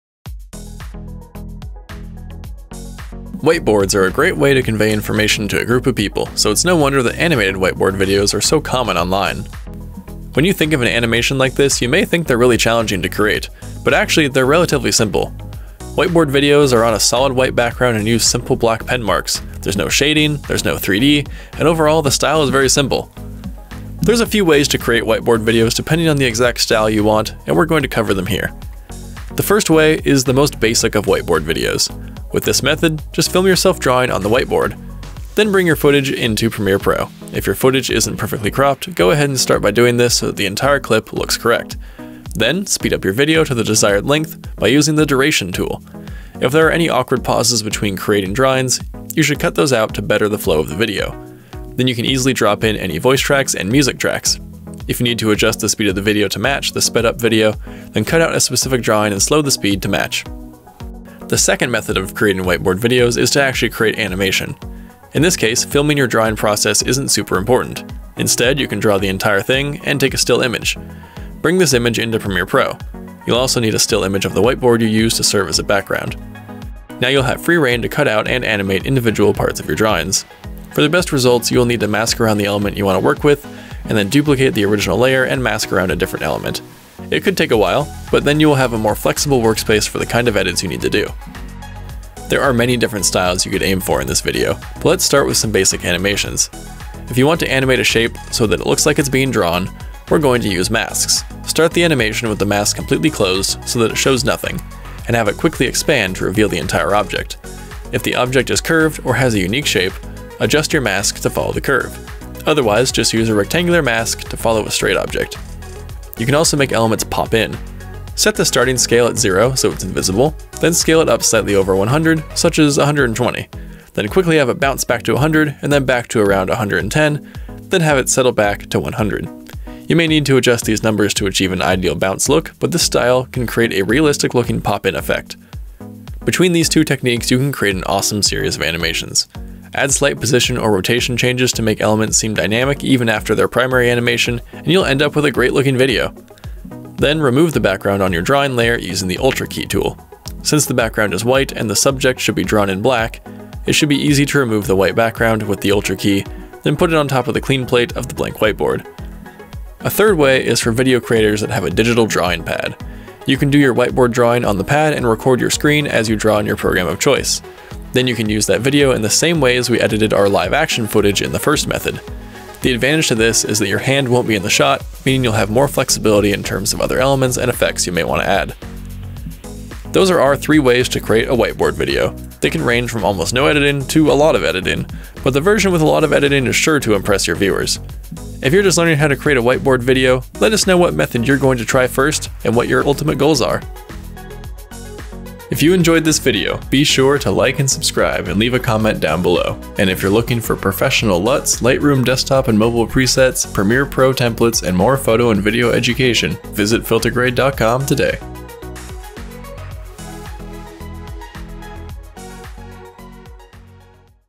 3 Simple Ways to Make Whiteboard Videos
Whiteboards are a great way to convey information, so it’s no wonder that animated whiteboard videos are so common online. When you think of animation like this, you may think they’re really challenging to create. But actually, they’re relatively simple! Whiteboard videos are on a solid white background and use simple black pen marks or colored markers - there’s no shading, there’s no 3D, and overall the style is very simple.

In this video, we're covering three simple ways to make whiteboard videos. Keep watching to learn more!

**All LUTs Tutorials**

Follow FilterGrade!